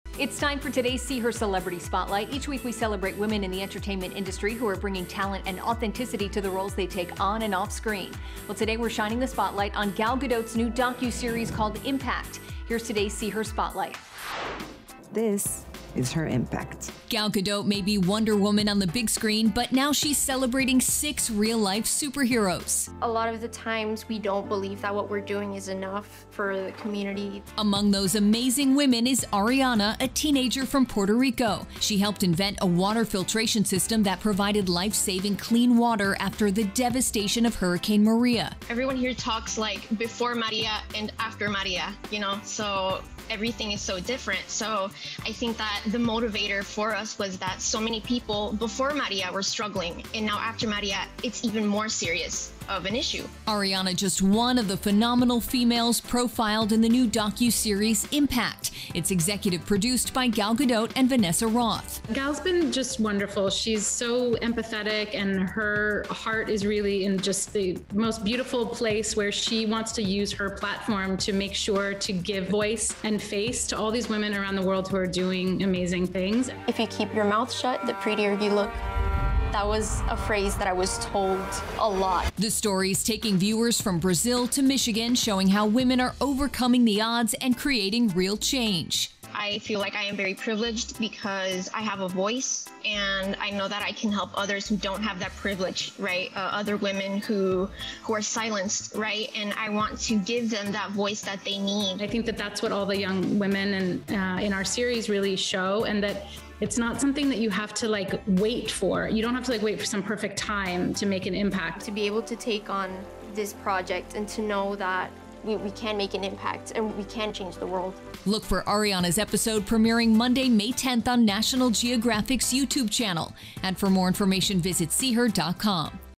Gal Gadot Makes An 'Impact' With Superhero Women All Over The World | Celebrity Page
"National Geographic Presents: Impact with Gal Gadot" is a docu-series that features GalGadot who presents the stories of six women creating real change in their corner of the world. This SeeHER Spotlight features one of the six, Arianna Font Martin, a student in Puerto Rico fighting the lingering effects of Hurricane Maria and the Executive Producer of the series, Vanessa Roth. The series is available on the NationalGeographic YouTube channel.